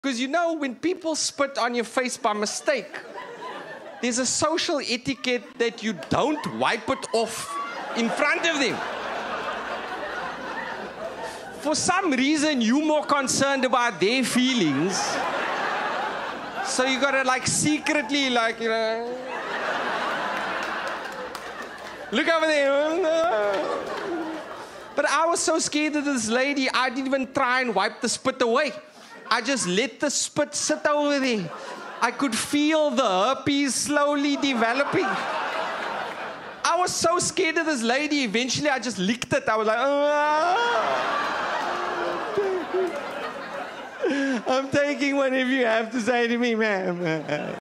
Flight attendant spat on my lip 💦😂 | Riaad Moosa | Standup Comedy
Have you noticed that when somebody spits on your face by mistake, you do something strange?